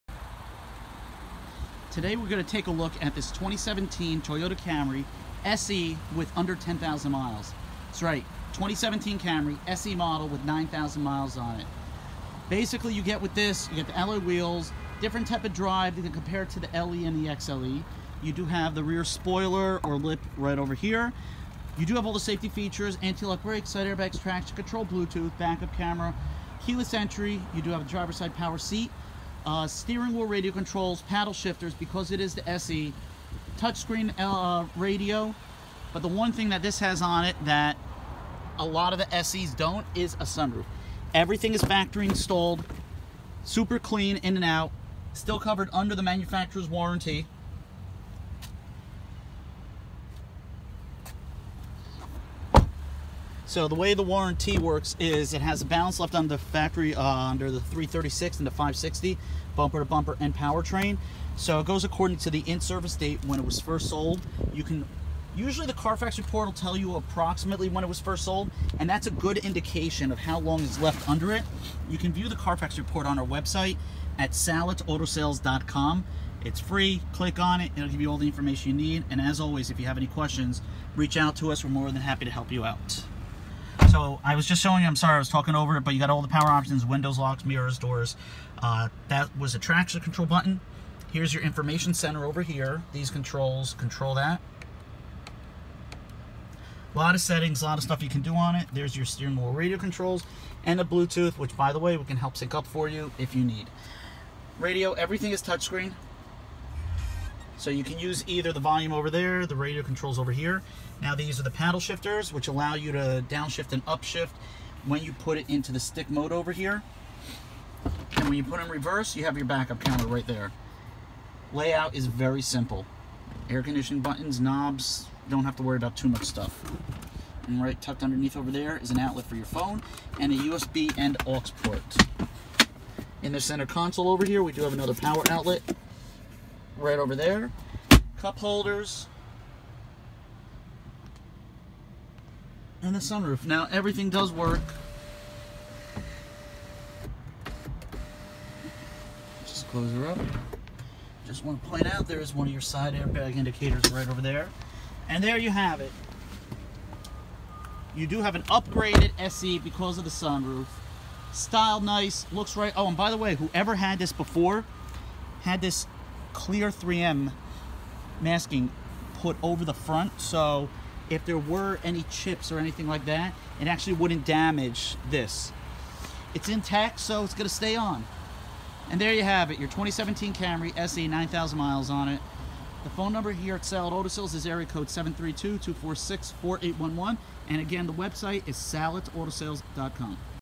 Salit Auto Sales - Gray 2017 Toyota Camry SE in Edison, NJ
This is a one owner 2017 Toyota Camry SE with 9,000 low miles on it. This Camry that is not a prior rental. It is certified by Carfax to be a one owner vehicle with a clean history report. It comes equipped with power windows, power door locks, ABS, side airbags, traction control, remote keyless entry, Toyota factory sunroof, bluetooth, backup camera, alloy wheels, driver side power seat and a remote keyless entry. Financing and extended warranties are available.

1855 Woodbridge Avenue
Edison, NJ, 08817